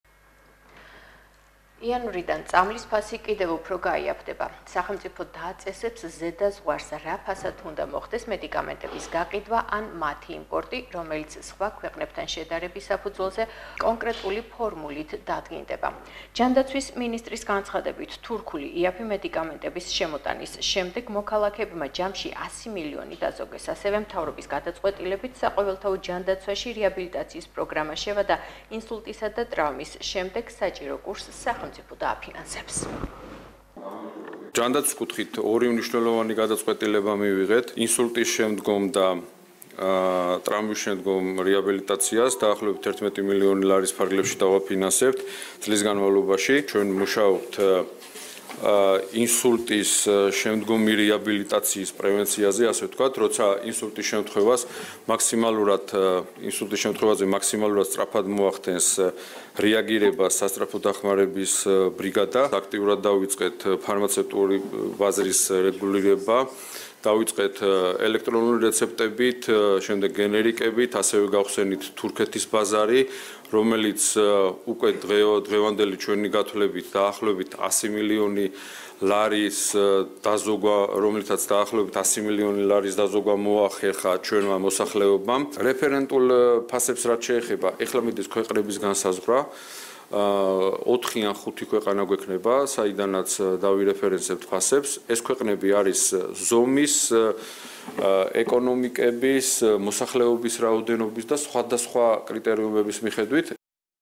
საყოველთაო ჯანდაცვის პროგრამით, სხვადასხვა დაავადებების შემდგომი რეაბილიტაცია დაფინანსდება9.11.2022
საყოველთაო ჯანდაცვის პროგრამით, ინსულტის, თავისა და ზურგის ტვინის ტრავმული დაზიანების შემდგომი რეაბილიტაცია დაფინანსდება